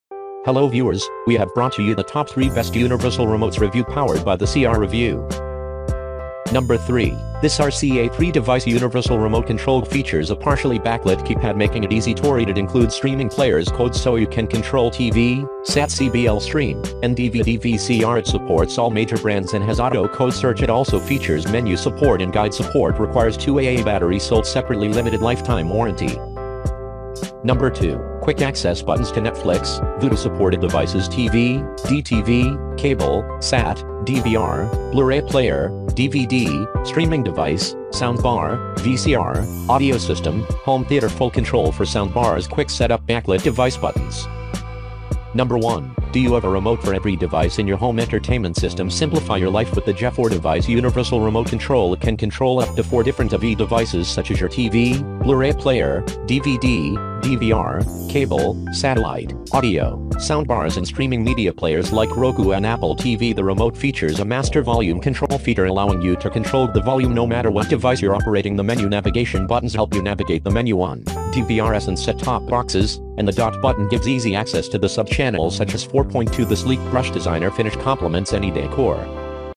Top 3 Best Universal Remotes Reviews in 2024
Get Discount From Here:
3.RCA RCR313BE
2.Philips SRP5018
1.GE 33709

If you're tired of all the clutter on your coffee table, or you waste half an hour every day with your head underneath the couch, then one of these universal remotes is for you. They can control your TV, sound bar, cable box, DVR, DVD player, and sometimes even streaming devices. Start spending more time watching and less time fumbling for the right controller.